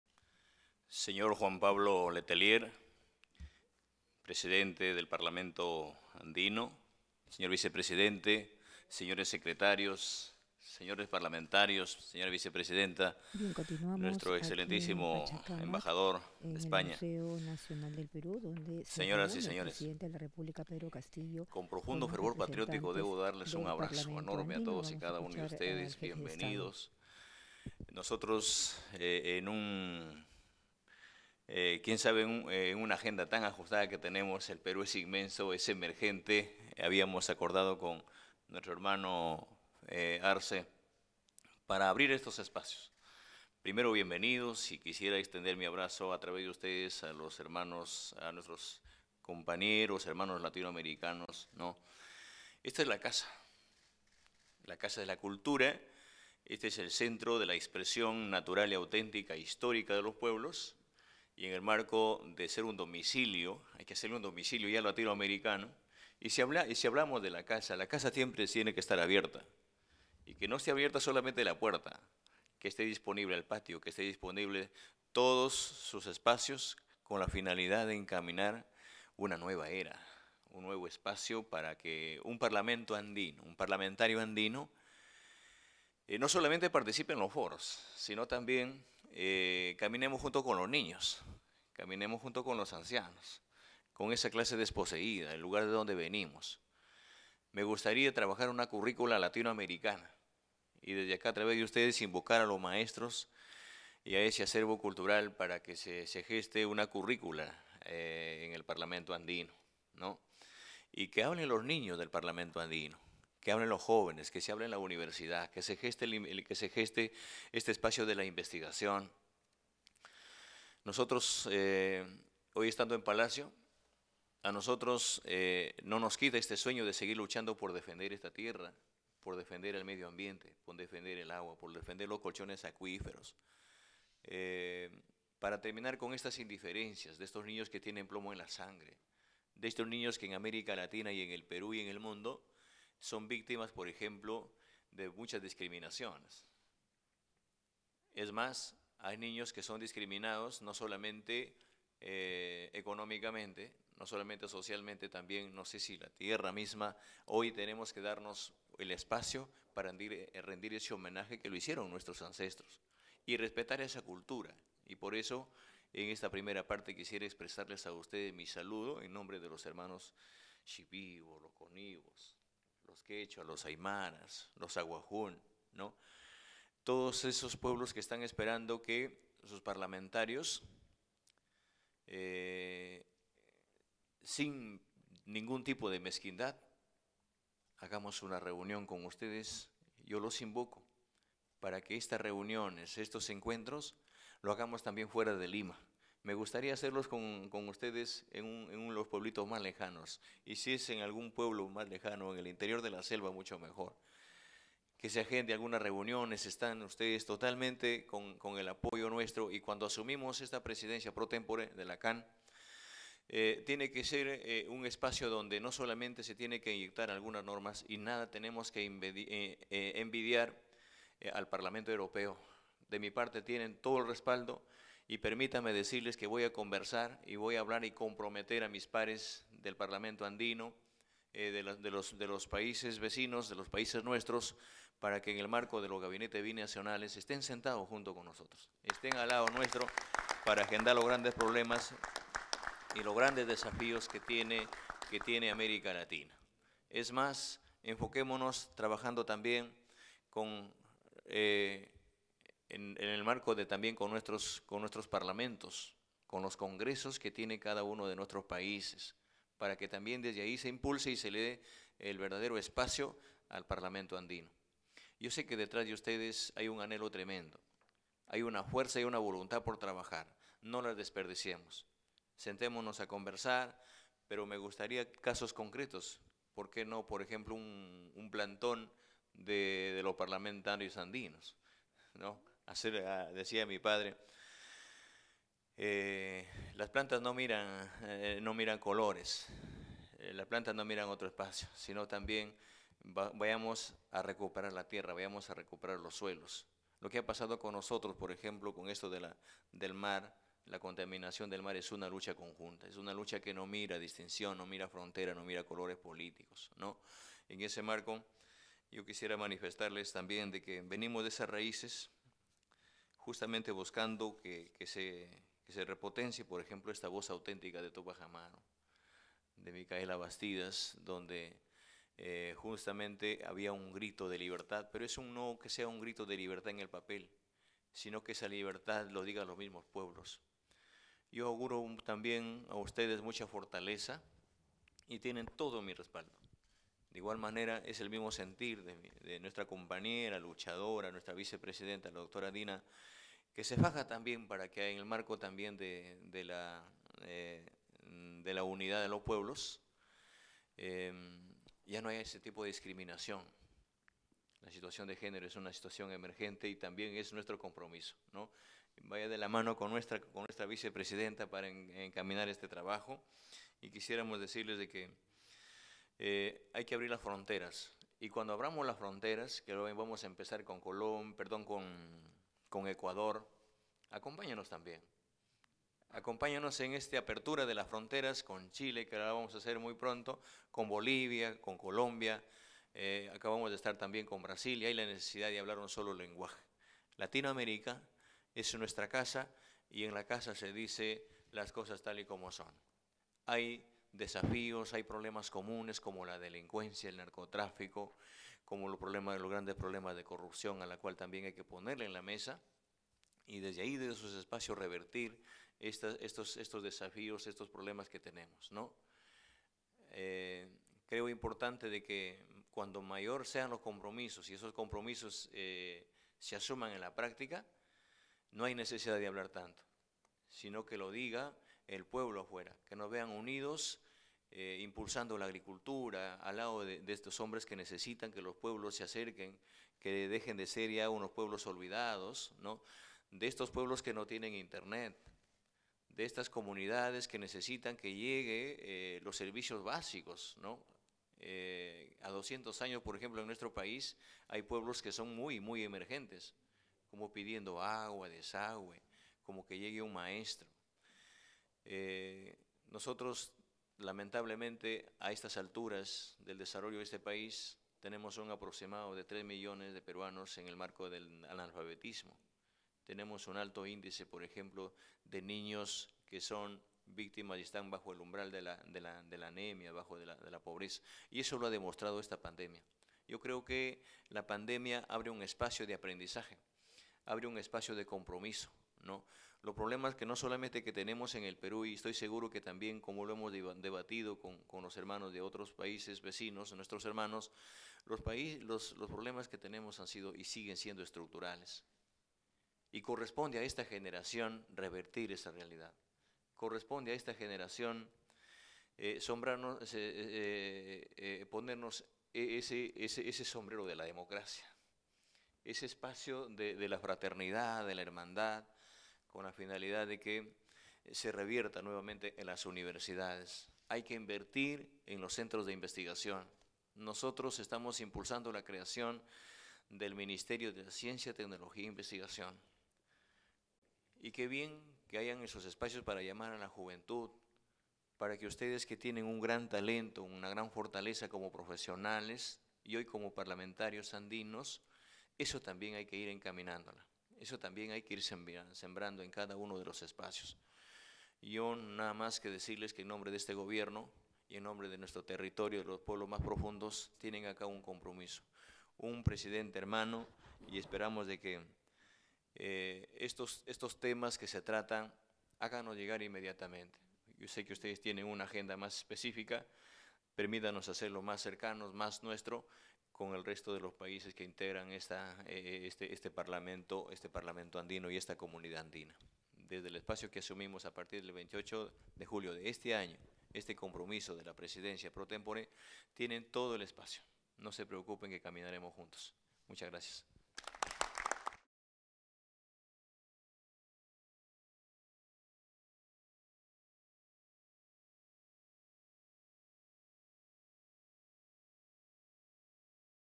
Lurín | Presidente Pedro Castillo se reúne con integrantes del Parlamento Andino
El presidente Pedro Castillo sostuvo una reunión con los integrantes del Parlamento Andino, en Lurín.